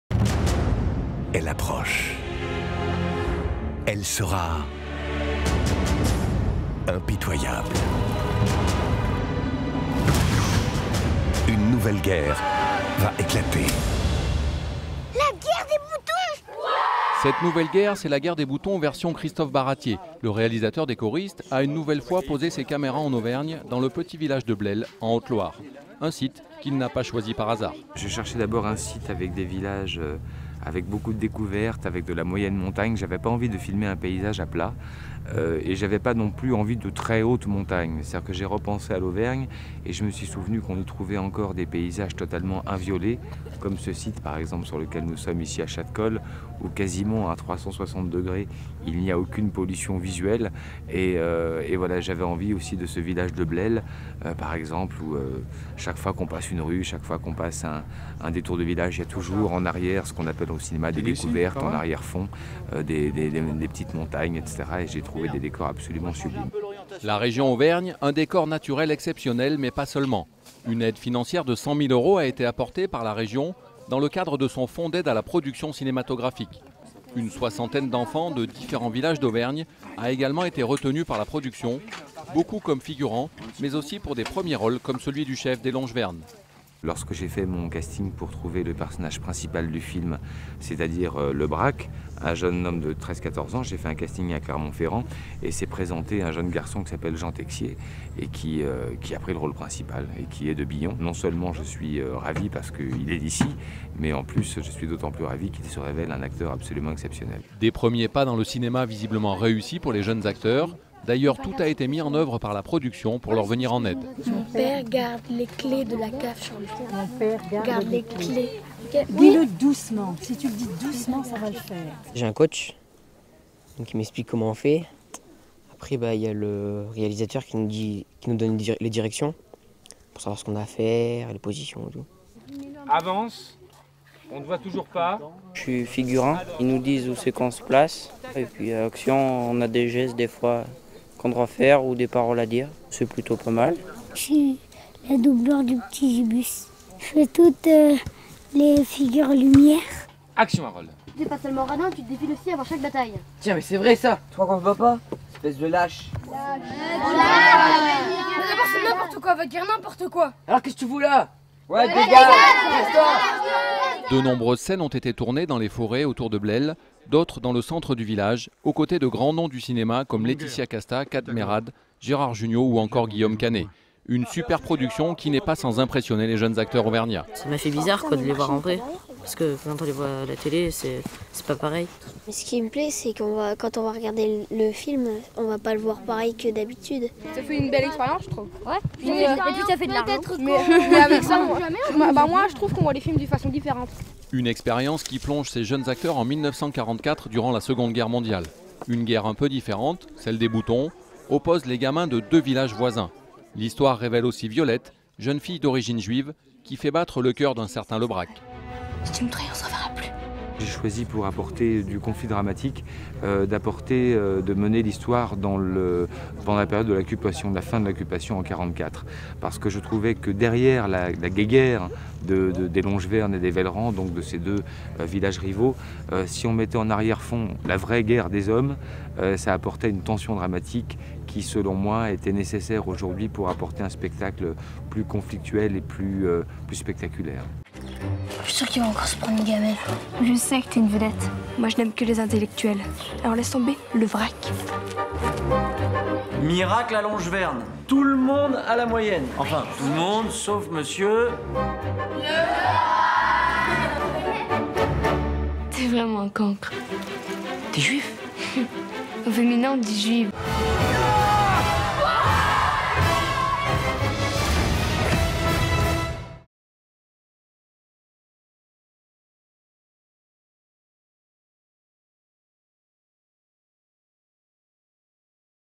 Auvergne, Terre de cinéma : La nouvelle guerre des boutons
En soutenant la Commission du Film Auvergne, la Région s'impose fortement comme une terre du 7e art. Le 21 septembre sort en salle La Nouvelle guerre des boutons, film de Christophe Barratier, tourné en Haute-Loire. A l'affiche des grands noms du cinéma français : Laetitia Casta, Kad Merad, Guillaume Canet et les enfants !